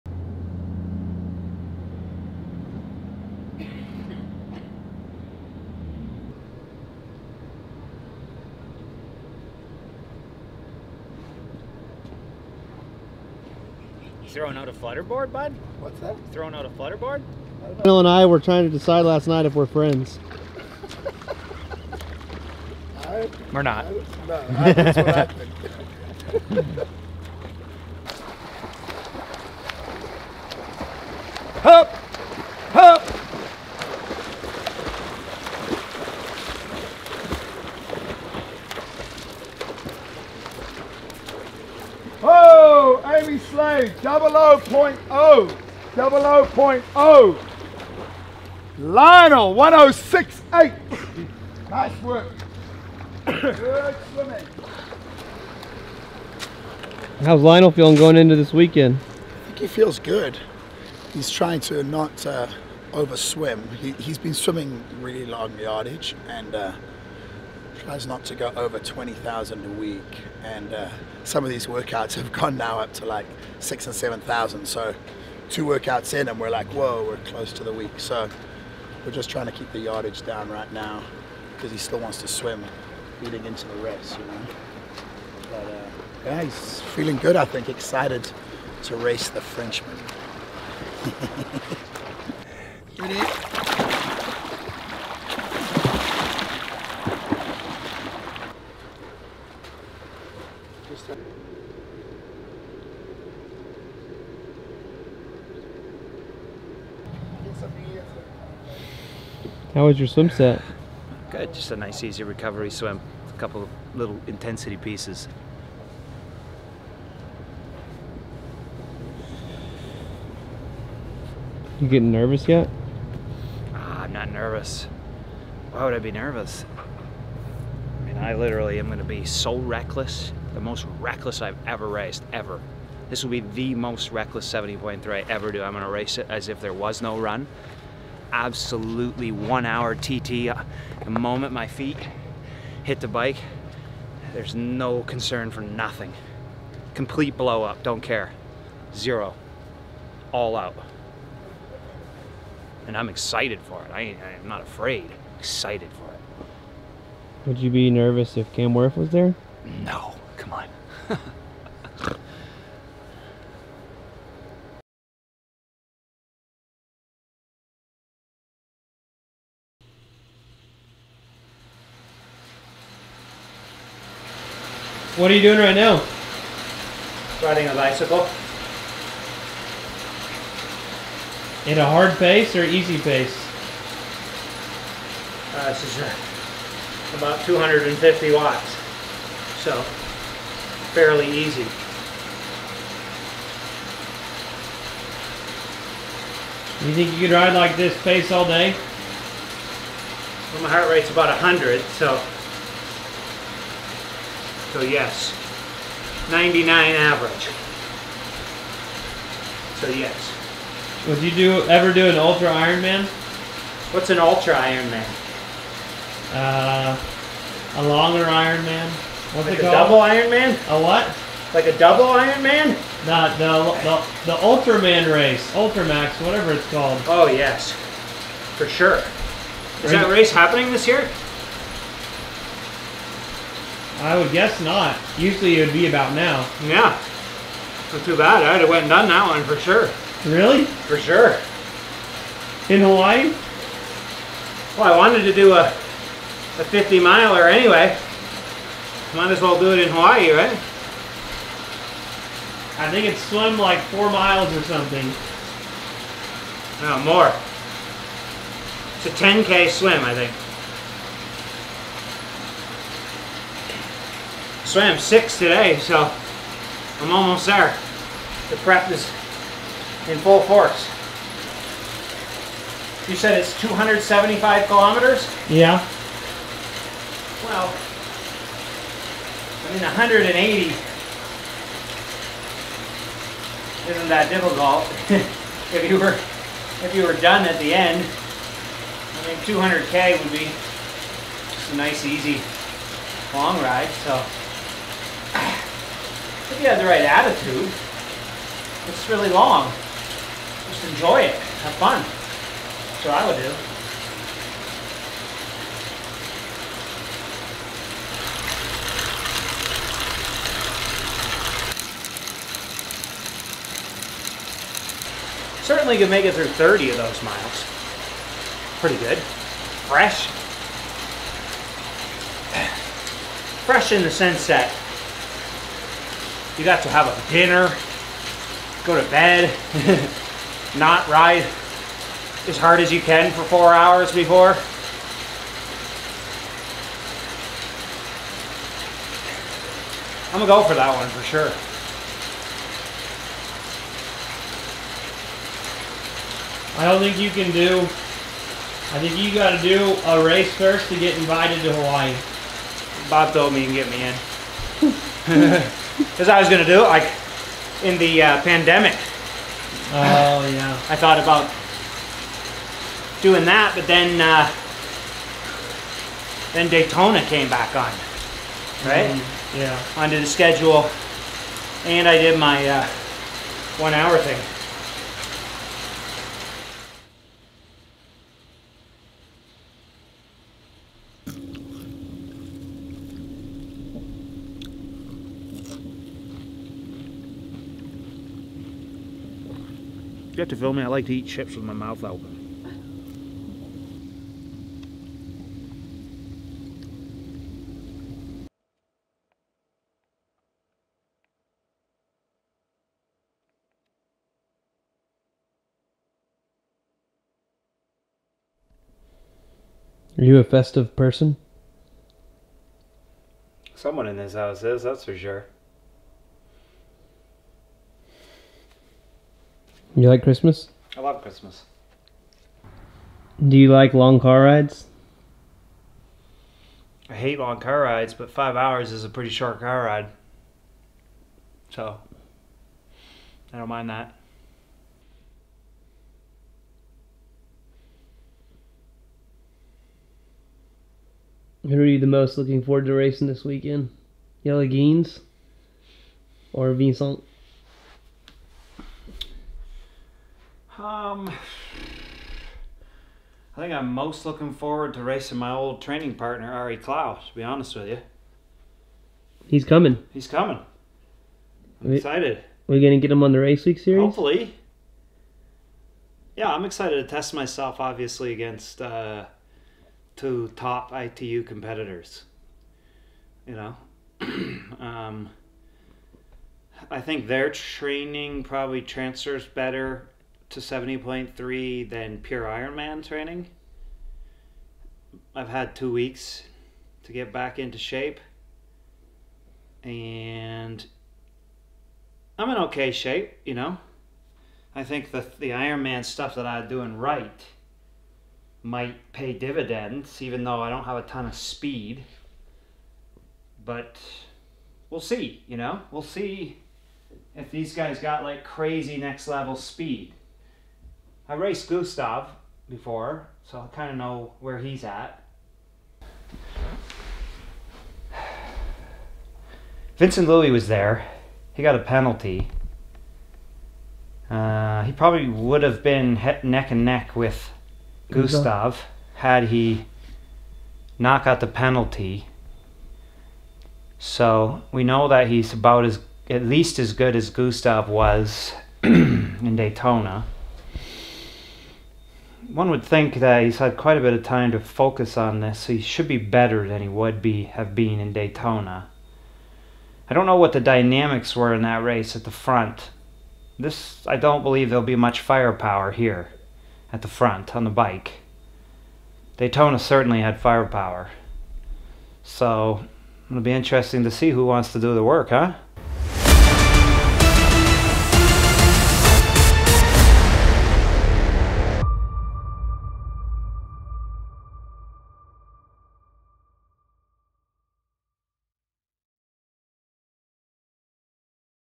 Ironman 70.3 Indian Wells: Race Week - Episode 1
It's "Race Week" This series is different from our original videos. These videos will have guests on them frequently and some of Lionel's competition. We plan to give you a more inside look at what goes on behind the scenes as well as build characters and help you meet other fellow pro athletes while getting you excited for this weekend's race.

Join us on the race week chatter on lionels Discord Server.

This race will not be broadcast. We will upload a post-race video as soon as possible.